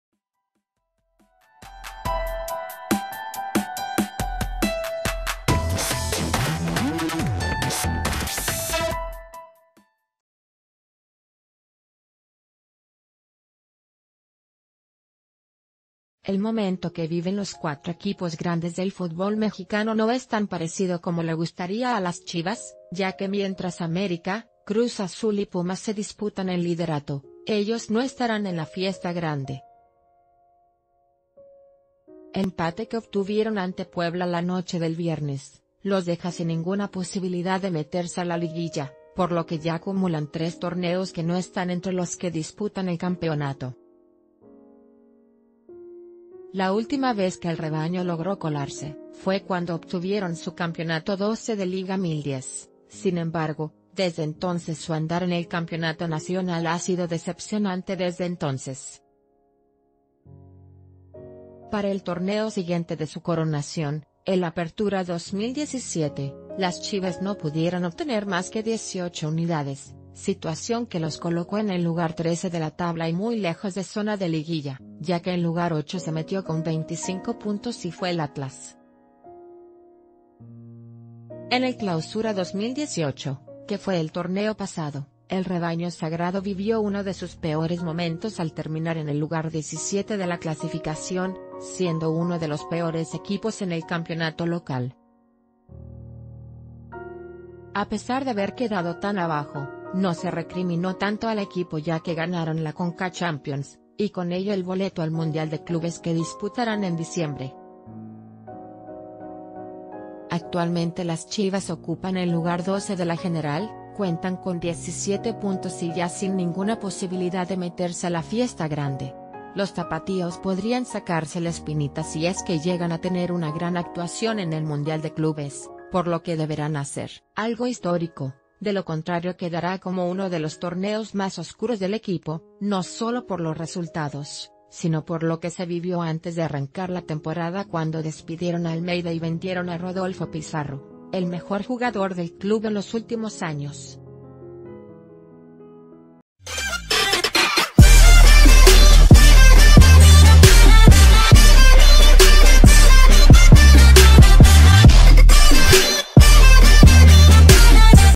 Chivas dice adiós a la Liguilla
El momento que viven los cuatro equipos grandes del futbol mexicano no es tan parecido como le gustaría a las Chivas, ya que mientras América, Cruz Azul y Pumas se disputan el liderato, ellos no estarán en la fiesta grande. El empate que obtuvieron ante Puebla la noche del viernes, los deja sin ning...